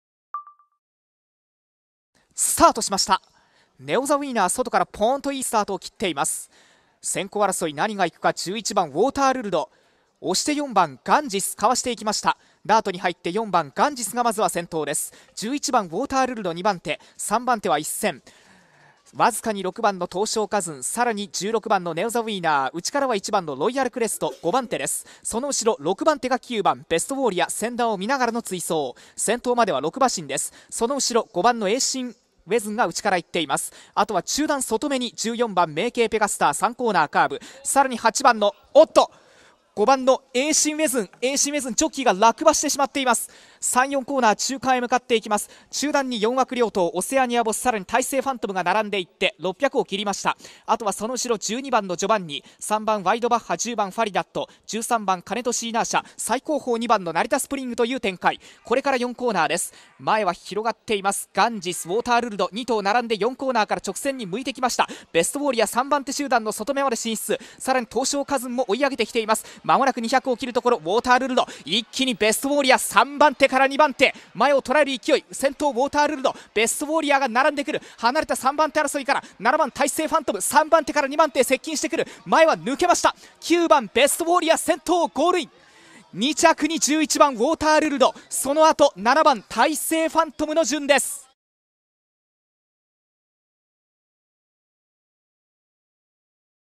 Best Warrior (KY) wins race 11 at Kyoto 2/8/2014
2/8/2014 Kyoto Race 11 Subaru Stakes 1400m
Best Warrior (pgm#9)

BEST WARRIOR (KY)
TB, CH, H, Foaled March 7, 2010
Majestic Warrior (KY)  - Flirtatious Miss (KY) , by Mr. Greeley (KY)
Breeder: Buck Pond Farm, Inc.